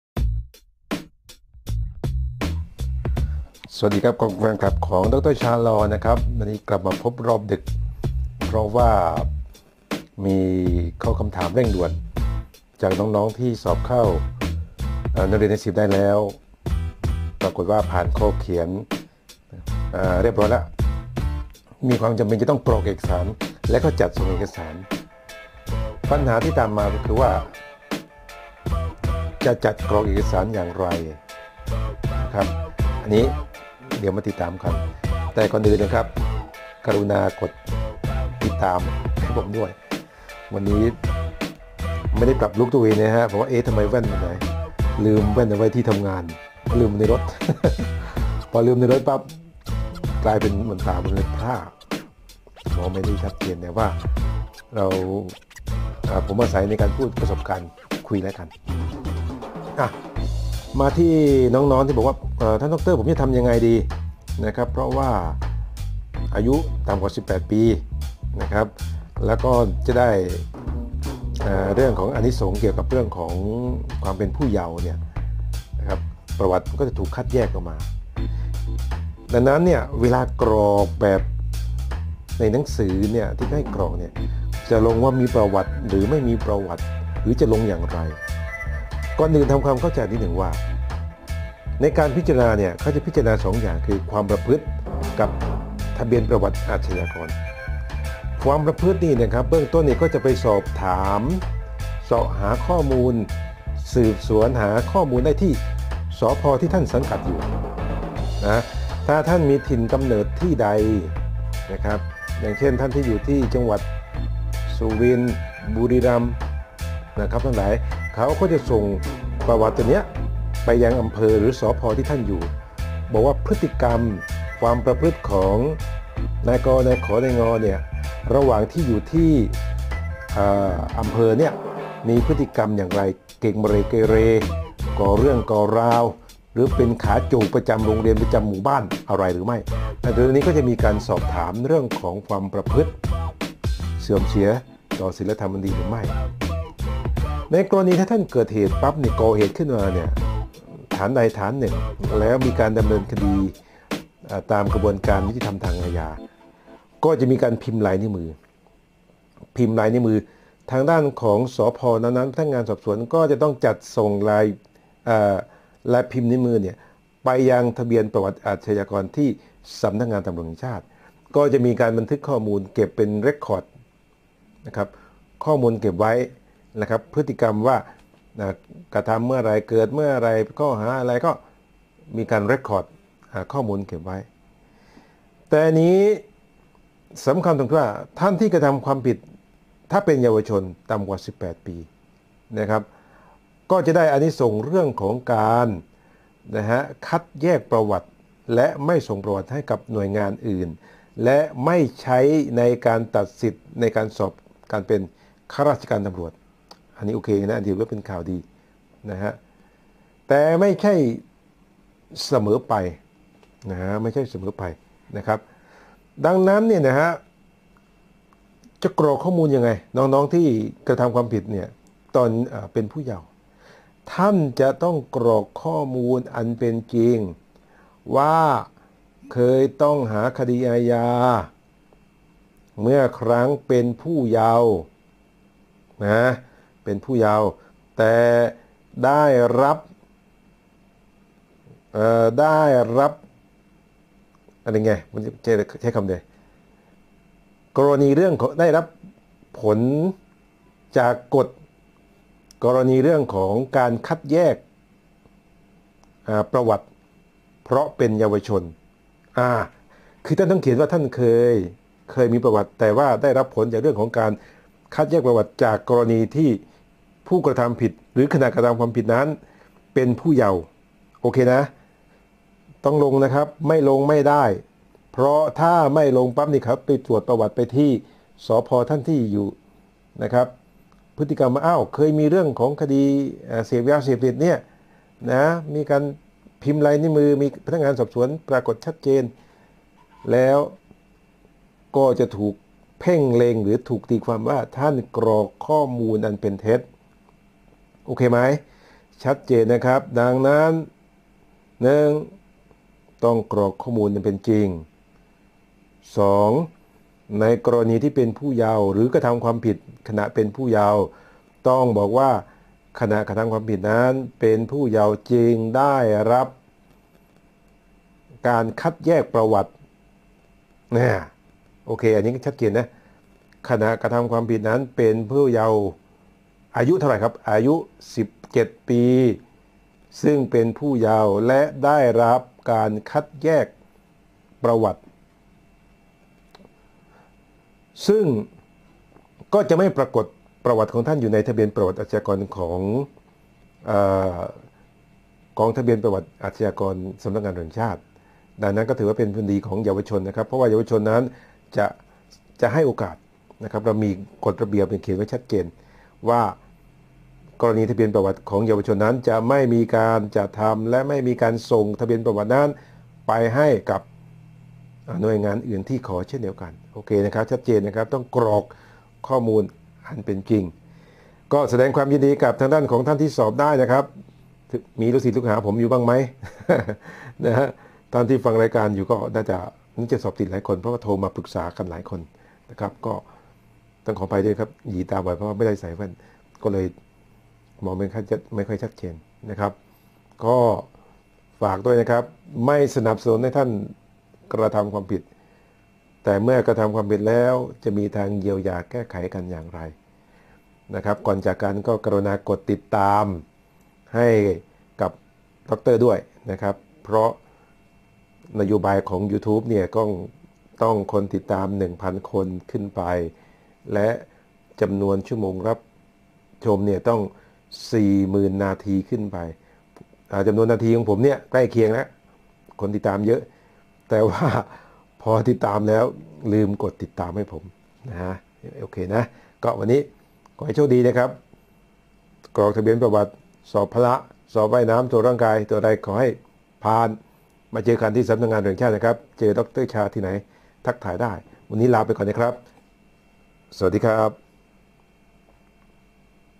ลอว์
ในการกรอกข้อเท็จจริงประวัติในรายงานตัว กรณีเคยต้องหาคดีอาญาควรจะกรอกตามความจริง ในกรณีตอนที่กระทำความผิด เป็นเยาวชนให้อ้างเรื่องการได้รับประโยชน์จากการคัดแยกประวัติแล้ว